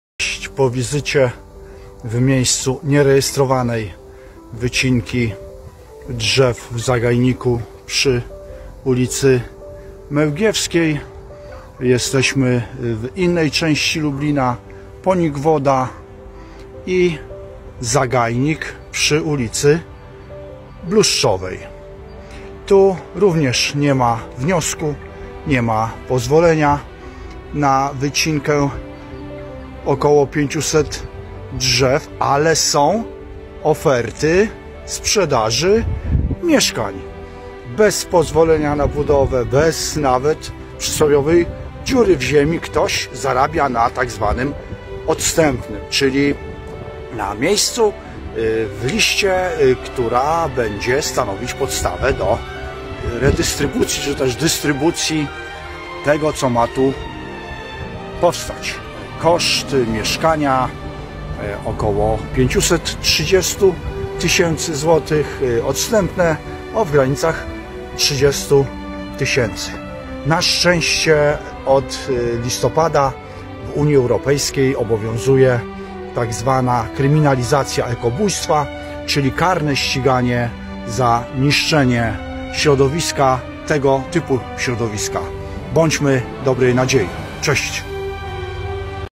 Zastanawialiście się kiedyś, jak daleko idą granice spekulacji, nieuczciwego handlu i kumoterstwa?
@💭🌳💣 Czy zastanawialiście się kiedyś, jak daleko sięgają granice spekulacji, nieuczciwego handlu i kumoterstwa?

W dzisiejszych czasach sprzedaż mieszkań na papierze to nowa fala posunięć, która szokuje nawet najbardziej doświadczonych obserwatorów rynku nieruchomości. I nie mówię tutaj o zwykłym inwestowaniu w przyszłość, ale o sprzedawaniu czegoś, czego jeszcze nie ma! 🏢😱

Aktualnie, mamy do czynienia z sytuacją, gdzie oferta sprzedaży lokali mieszkalnych w blokach planowanych na miejscu jednego z naszych miejskich zagajników została upubliczniona. Ale uwaga! Nie istnieje żaden wniosek ani pozwolenie na wycinkę rosnących tam drzew, a co dopiero na rozpoczęcie budowy. ⛔️🌳🚧

To jest coś znacznie poważniejszego niż zwykła spekulacja. Takie działania wywołują nieuzasadnione pragnienie posiadania czegoś, co w rzeczywistości nie istnieje. Ludzie sięgają więc po pozaprawne środki, aby wywiązać się z przyjętych zobowiązań finansowych za tzw. odstępne. To prowadzi do sztucznego podwyższania cen i wysycania zapotrzebowania. Efektem tego jest nieuzasadnione napędzanie koniunktury i osiąganie wysokich zysków. 💰🔝

Niestety, patologia związana z mieszkalnictwem, która jest akceptowana przez organy samorządu i państwa, oraz część społeczeństwa, uderza przede wszystkim w mniej zamożną część obywateli. Ta sytuacja sprzyja także tragicznej wręcz depopulacji nie tylko Lublina, ale całej Polski. 🏚️😔

Dlatego właśnie, aby powstrzymać to szaleństwo, jedynym rozwiązaniem może być przyjęcie kryminalizacji niszczenia takich miejsc jak ten zagajnik. Sankcje za takie działania, czyli ekobójstwo, powinny stać się nie tylko moralnym nakazem, ale również prawną odpowiedzialnością. Ochrona naszej przyrody i dbanie o przyszłe pokolenia muszą być naszym priorytetem. 🌳❤️

I w tym momencie dochodzimy do morału tej historii. Jeśli nieustannie pozwalamy samorządowi i rządowi na takie działania, to sami stajemy się winni. Tylko poprzez wprowadzenie takich sankcji, jakie przyjęto w Unii Europejskiej o kryminalizacji niszczenia środowiska naturalnego (ekobójstwo), możemy zakończyć tę haniebną sytuację. Czas się obudzić i zadbać o nasze miejsca, nasze drzewa i naszą przyszłość. I nie chodzi oczywiście o karanie, a świadomość możliwej odpowiedzialności!!!🌱💪

Poprzedni post w tej kwestii trywializowany przez niektórych uczestników tego forum spowodował, że nie tylko ta oferta jest już nieaktywna i zniknęła jak kamfora razem z jej wystawcą:
"Mieszkanie składa się z dwóch sypialni, salonu połączonego z kuchnią, przedpokoju, łazienki, wc i balkonu. Jest możliwość wyodrębnienia osobnej kuchni z oknem. Blok nr 1. Do mieszkania oddzielny 19m² zamykany podziemny garaż w cenie 42000zł oraz pomieszczenie gospodarcze 6m² w cenie 10000zł. Cena za mieszkanie 529000zł. Termin oddania koniec 2025 r. Po więcej informacji zapraszam prywatnie. Tylko 3 niskie 3 piętrowe bloki w otoczeniu domów jednorodzinnych.
ulica Bluszczowa 2a, boczna Dożynkowej".